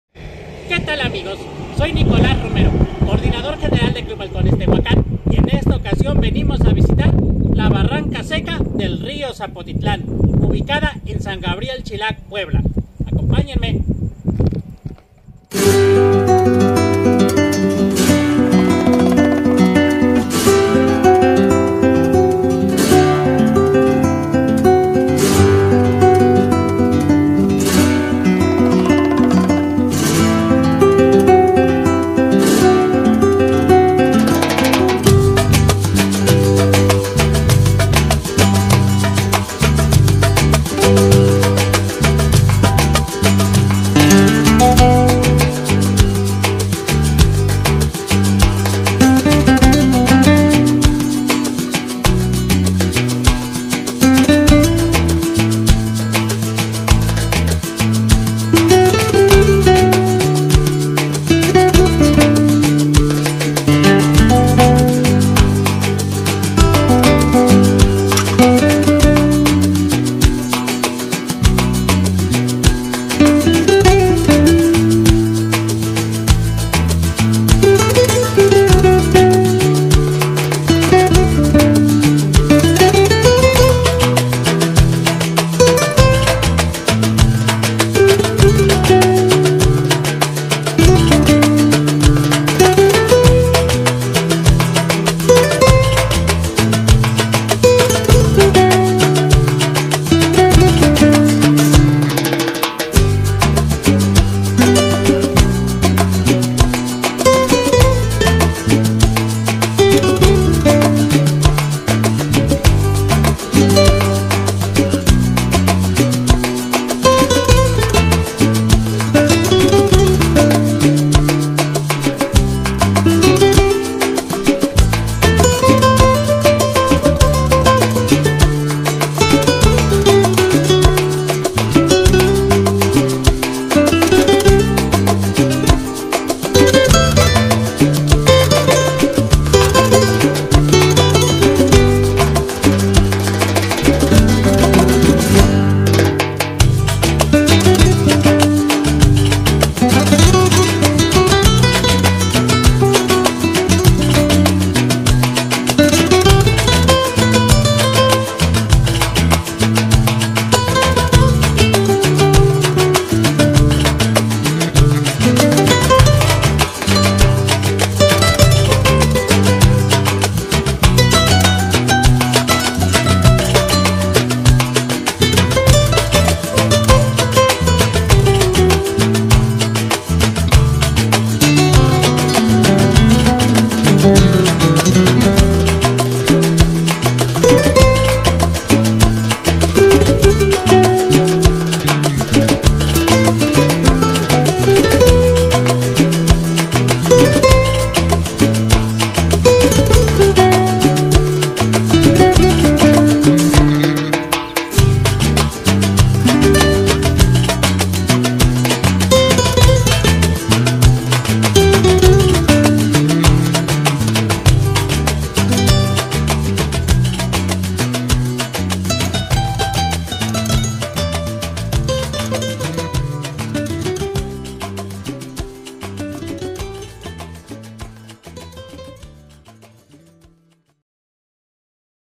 🌵Río Zapotitlan 🌤️San Gabriel Chilac🌴Puebla🐍- Club Halcones Tehuacán Puebla, Mx.
El Río Zapotitlán , nace en el corazón de la sierra de Zapotitlán Salinas, Puebla y recorre el valle de Tehuacán en el borde  cruzando por territorio de los municipios de San Gabriel Chilac  y San Jose Miahuatlán  , Puebla , hasta unirse al Río El Salado en Coxcatlan , Puebla.

Hoy vamos a conocer una pequeña sección la cual pertenece a la de San Gabriel Chilac , cabe resaltar que el Río Zapotitlan en siglos pasado corría por las lluvias más constantes y el brote de varios manantiales a lo largo de su barranca , actualmente solo es de Temporal , es decir , su cause solo se origina por las lluvias , aunque en algunos tramos aún sigue brotando ojos de agua que dan origen a algunas pozas y estanques .🌴🌤️🌵